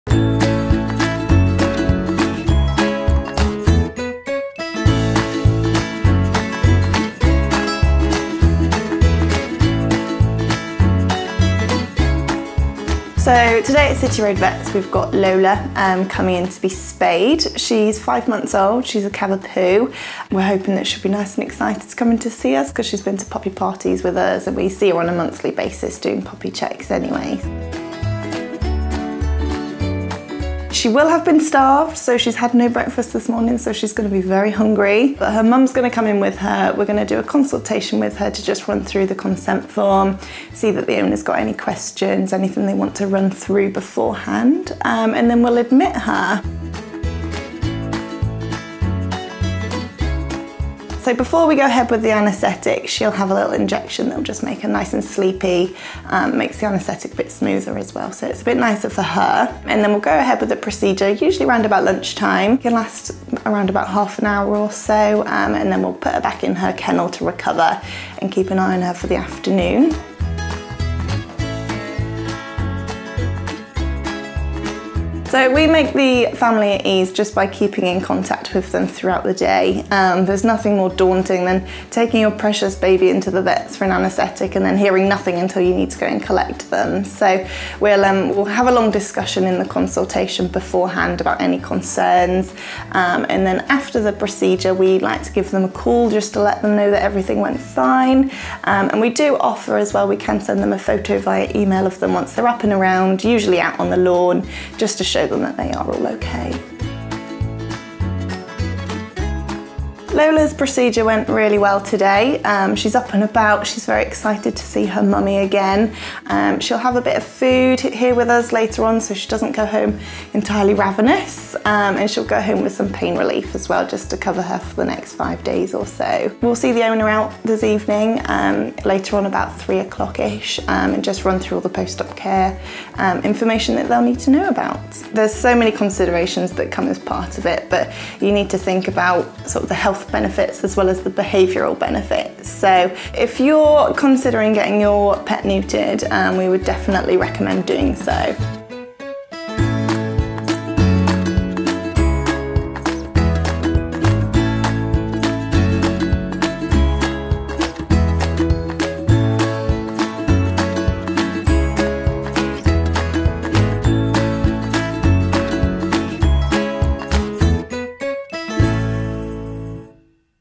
Neutering Your Dog | City Road Vets
We know that choosing to neuter your pet leaves you with a lot to consider. We hope that in this video you can have a better understanding of the benefits of doing so and get an idea of the process, as we follow head veterinary nurse, Becky as she looks after 'lola'.